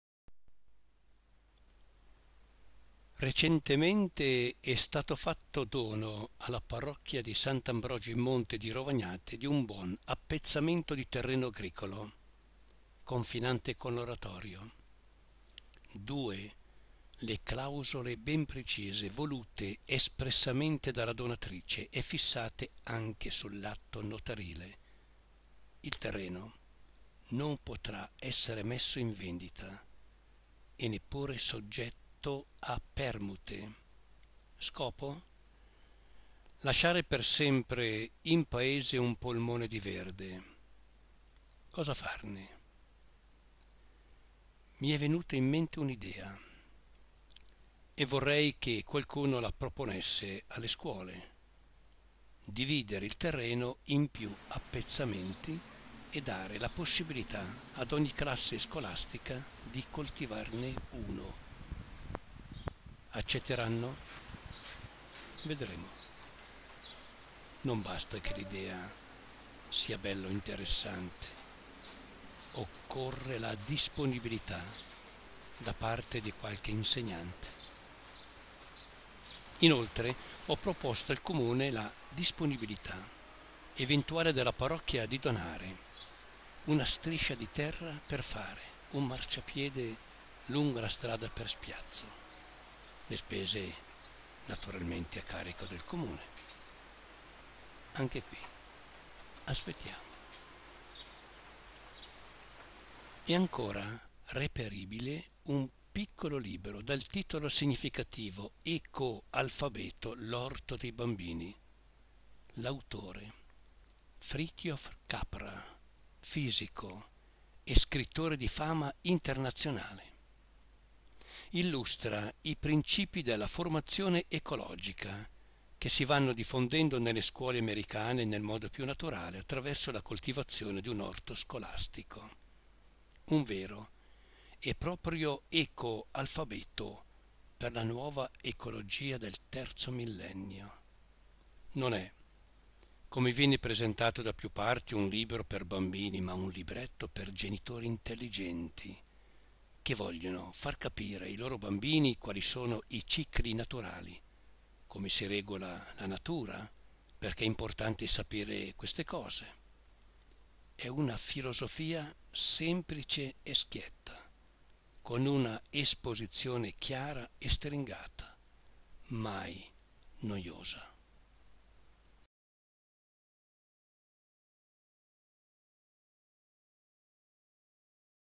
Orto botanico: una proposta per le scuole.MPG
Dare la possibilità alle classi scolastiche di coltivare un orto botanico. Lo propone anche Fritjof Capra in un suo prezioso libretto: "Ecoalfabeto - L'orto dei bambini".